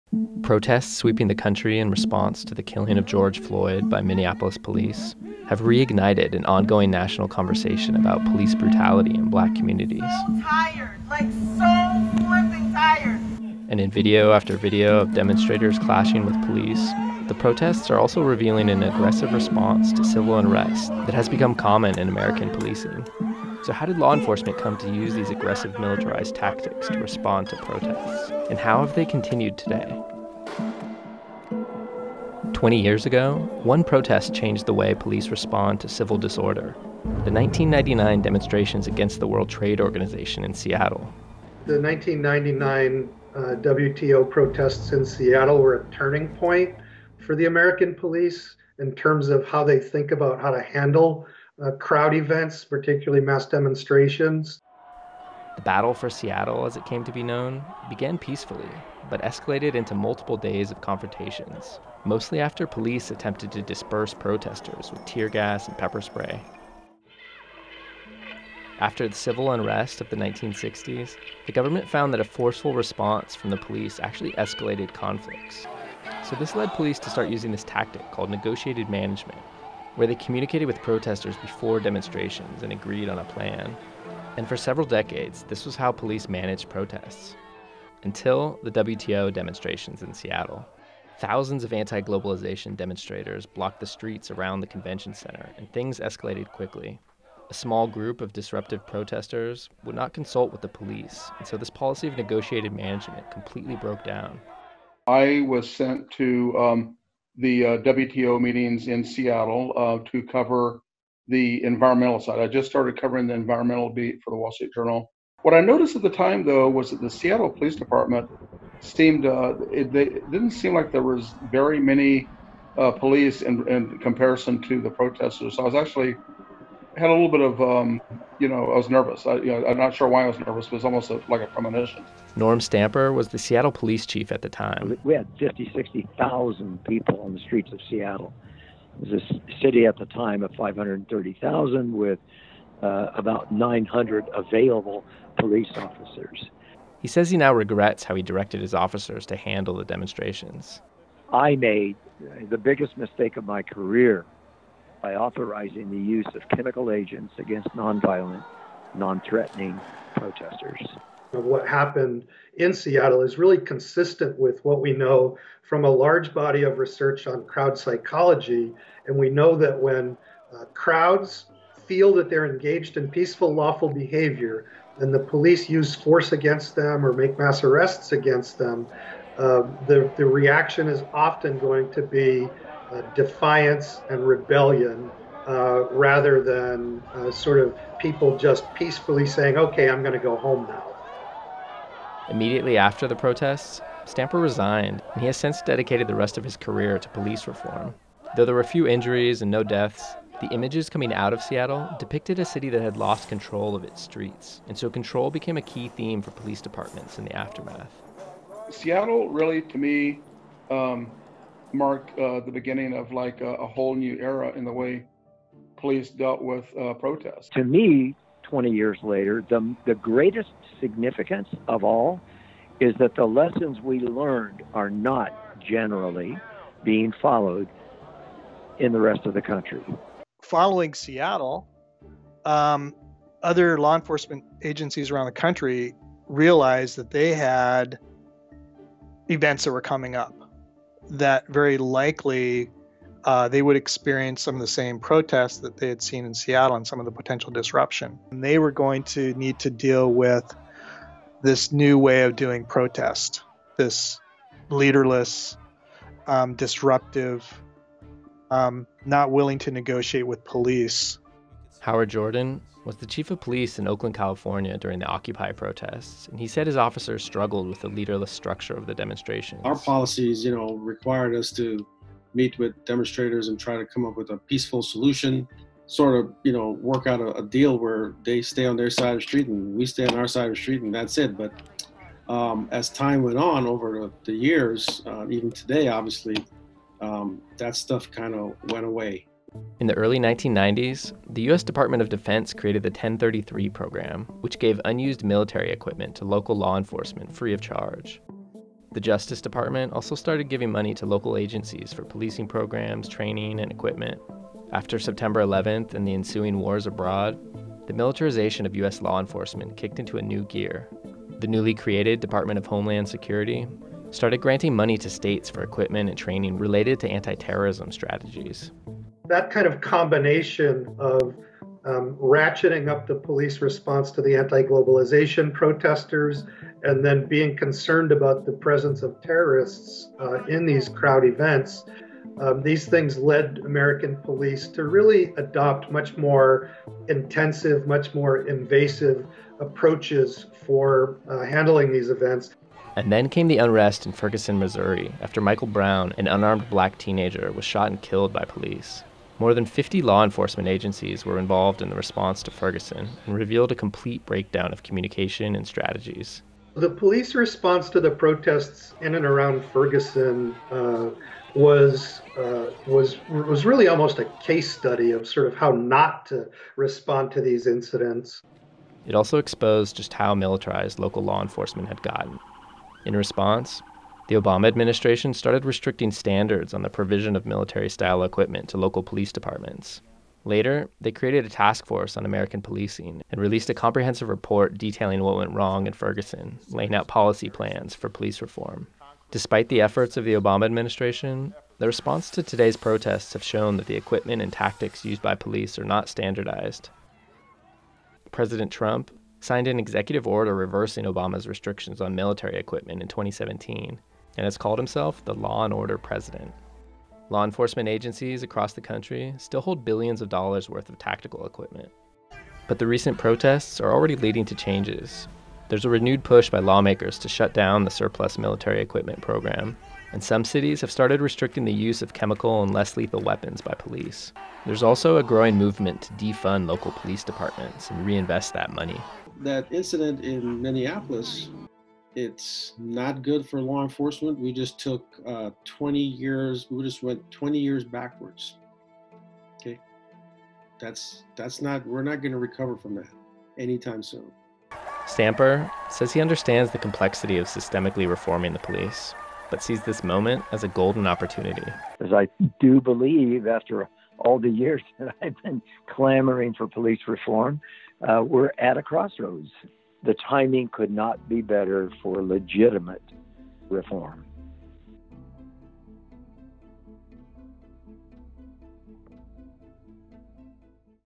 How 1999's WTO Protests Influenced the Policing of Protests Today | WSJ
Violent confrontations between police and protestors during the 1999 World Trade Organization conference changed the way police respond to protests in the U.S. Here’s how we got to the militarized police tactics we see today. Eric Draper/Associated Press